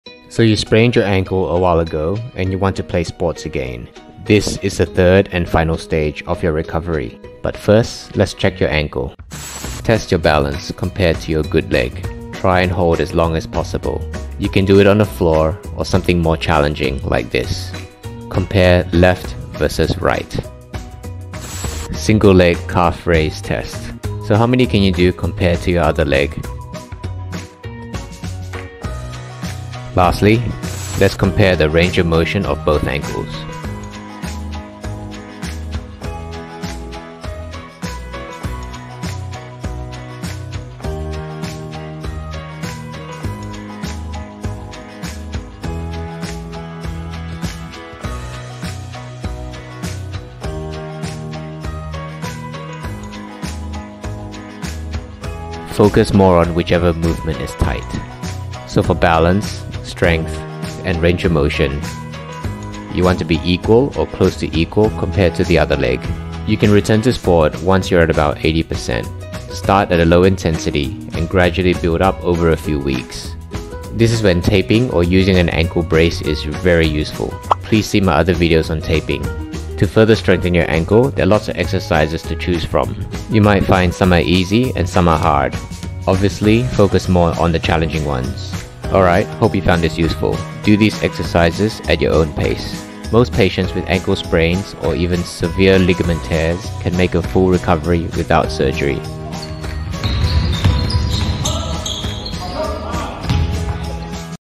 I Just Sprained My Ankle: What To Do (part 3)
So this is the final stage of ankle rehab. You are now able to walk pain free, but you might feel the ankle is abit stiff,  unstable and weak. Let's get you ready for Sports! This is the 3rd of my 3 part series on Ankle Sprains.